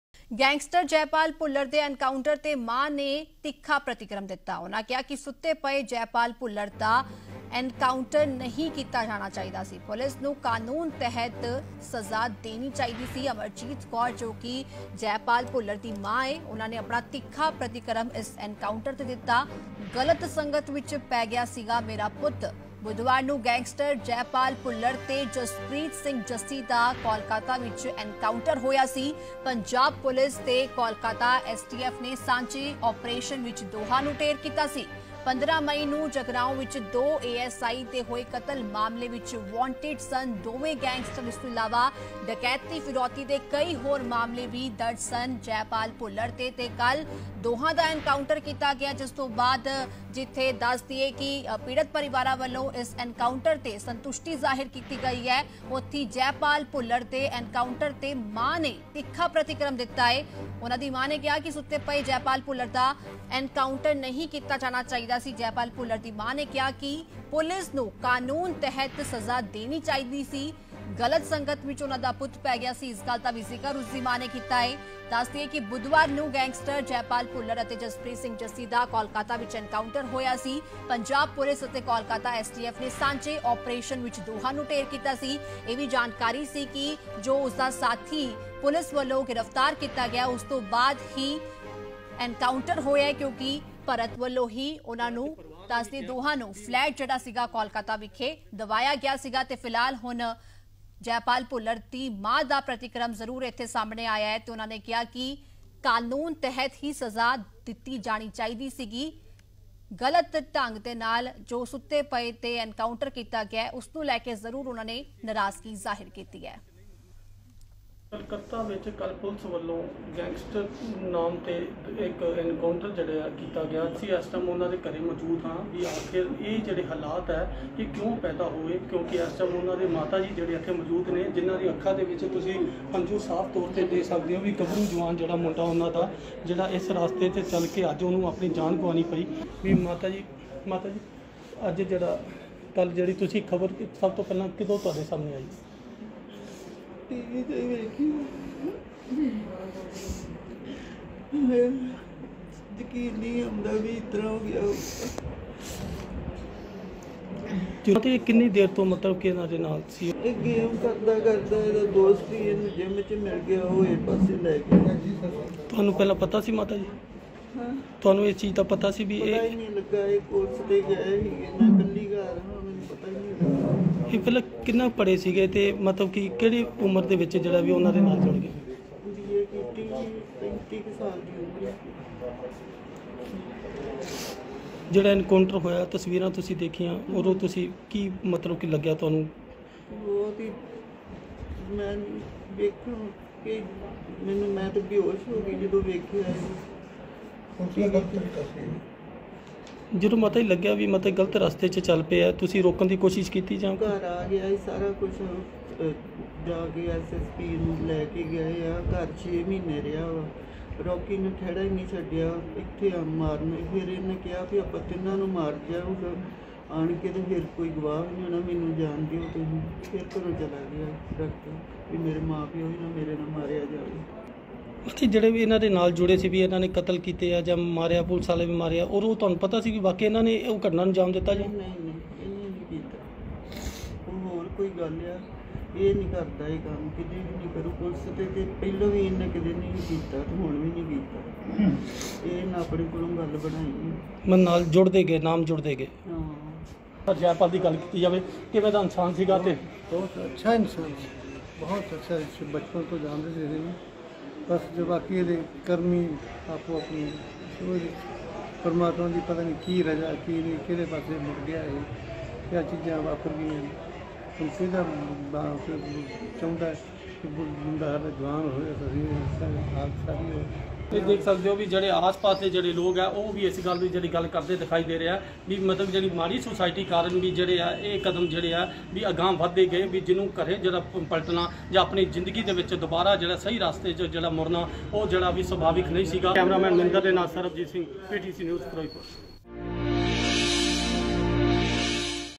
Encounter ਤੋਂ ਬਾਅਦ Jaipal Bhullar ਦੀ ਮਾਂ ਦਾ ਆਇਆ ਤਿੱਖਾ ਪ੍ਰਤੀਕਰਮ| Jaipal Bhullar Kolkata Encounter
In a major crackdown, Punjab’s most wanted gangster Jaipal Singh Bhullar and one of his aides have been killed in an encounter near Kolkata on Wednesday.